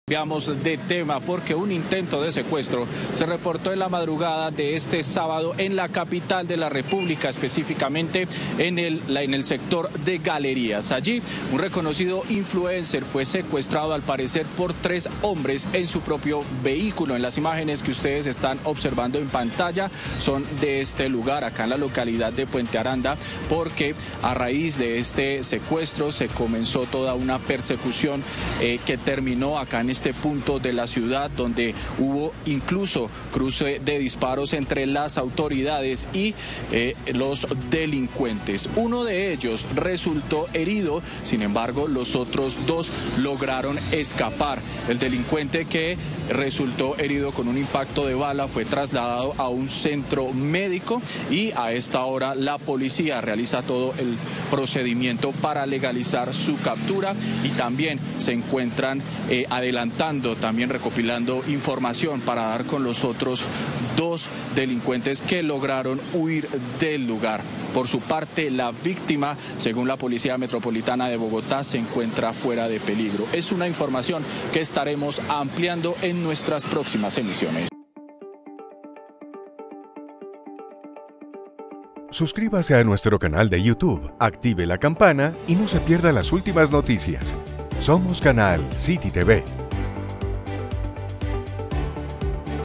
Influencer fue víctima de intento de secuestro en Galerías | CityTv
En las últimas horas, un reconocido influencer fue víctima de un intento de secuestro en su propio vehículo por parte de tres sujetos en el barrio Galerías. En medio del incidente, las autoridades se percataron dando origen a una persecución y a un cruce de disparos.